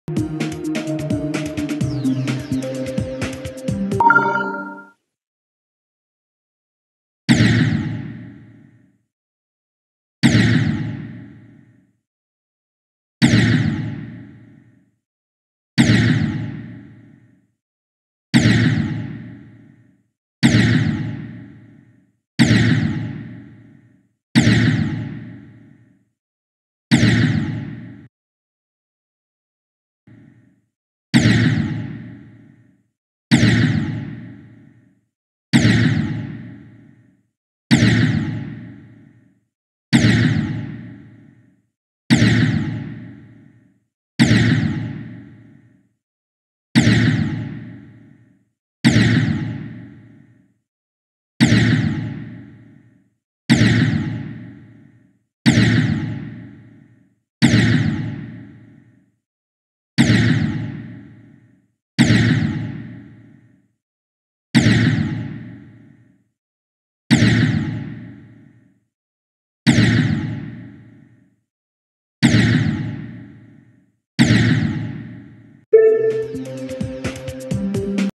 Is this level even possible?
I attempted to try and beat this level, but I thought this level was impossible because of the other player not doing anything.